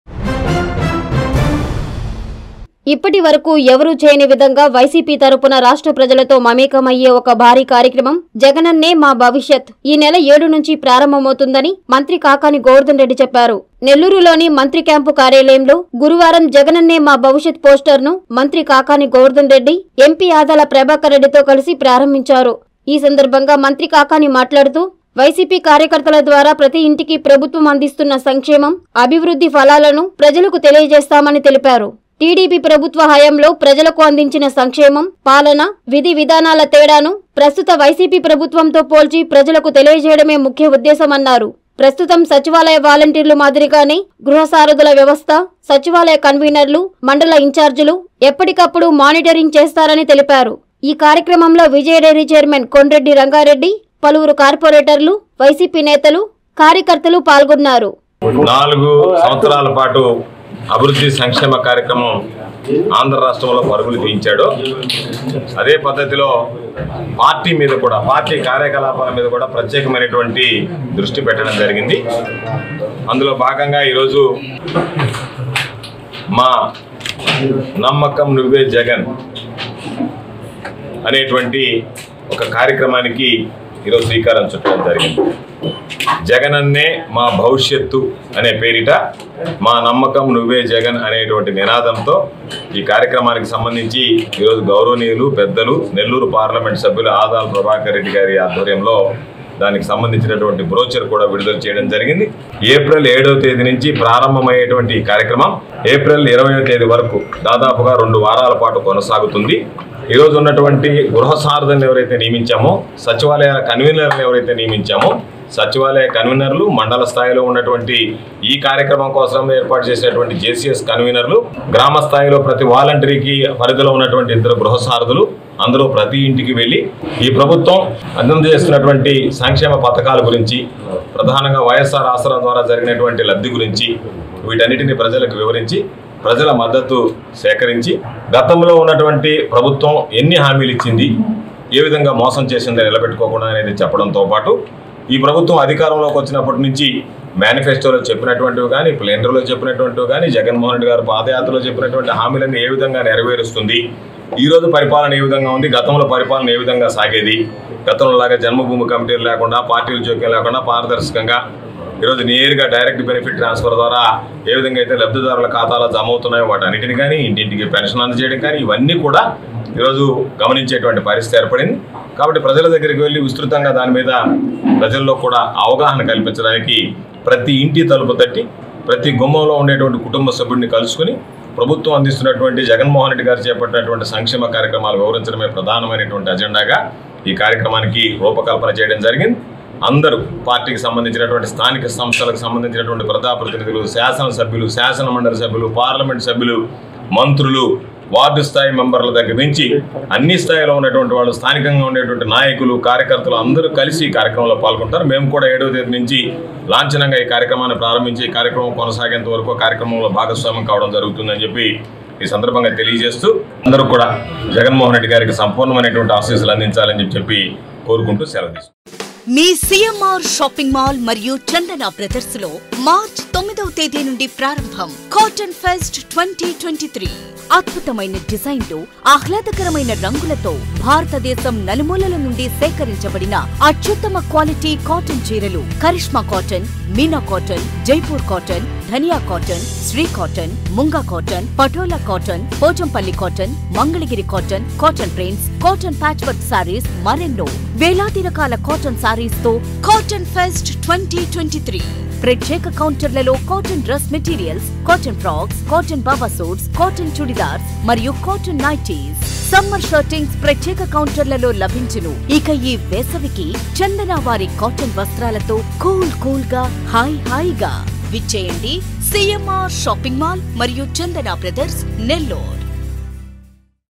7 నుంచి ‘జగనన్నే మా భవిష్యత్తు’
7 నుంచి ‘జగనన్నే మా భవిష్యత్తు’
14 రోజుల పాటు ఇంటింటి ప్రచారం
గత, ప్రస్తుత ప్రభుత్వాల పాలనలో వ్యత్యాసాలు వివరిస్తాం
నెల్లూరు క్యాంప్ ఆఫీస్‌లో పోస్ట‌ర్ ఆవిష్క‌రించిన మంత్రి కాకాణి, ఎంపీ ఆదాల‌